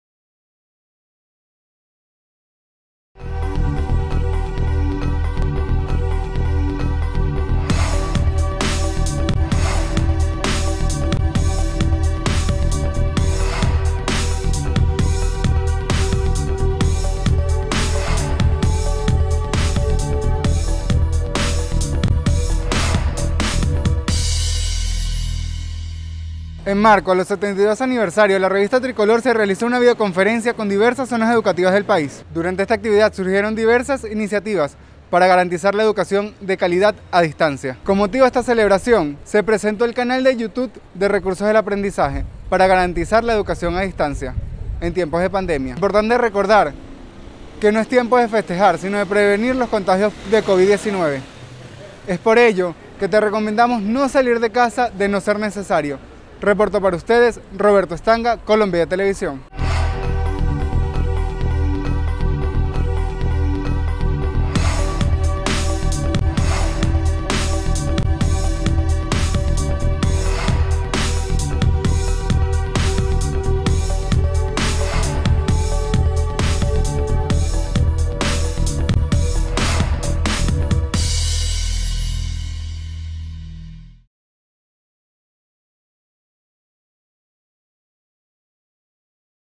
72 ANIVERSARIO DE LA REVISTA TRICOLOR
Revista Tricolor arriba a 72 años de conocimiento y alegría. Tricolor es una revista escrita con amor en cada letra; es como una niña y un niño, siempre joven; es un orgullo de la infancia venezolana y el alma de todas las generaciones.

La revista es una importante herramienta educativa que forma parte del Sistema de Recursos para el Aprendizaje del Ministerio del Poder Popular para la Educación, refiere una nota de prensa.

Este 24 de marzo, Tricolor cumple 72 años llevando educación y alegría a los estudiantes de la Patria Bolivariana. Desde su creación el 4 de febrero de 1949, Tricolor ha recogido el decir de la Patria y ha apoyado a los estudiantes del país, que encuentran un recurso de aprendizaje completamente dinámico.

Los estudiantes, las maestras, los maestros y la familia celebran con alegría el aniversario 72 de la revista que les ha permitido acceder a contenidos históricos, científicos, literarios, culinarios, sobre fauna venezolana y otros temas que enriquecen el proceso de construcción educativa de niñas, niños y jóvenes.

Tricolor es una opción educativa, recreacional y de formación de valores, que promueve la curiosidad, el conocimiento, la investigación, la memoria histórica nacional, regional y local.

Es un recurso para la exploración lúdica, la reafirmación de identidad con la Venezuela que se construye con saberes y haceres, que cada mes ofrece diversas posibilidades para apoyar las actividades en el aula, la biblioteca y el hogar.

Es un bien de interés cultural pues fue declarada Patrimonio Cultural de Venezuela el 30 de marzo de 2019. Está amparada por la Ley de Protección y Defensa del Patrimonio Cultural, por ser reconocida como una revista que imparte educación y cultura en el país.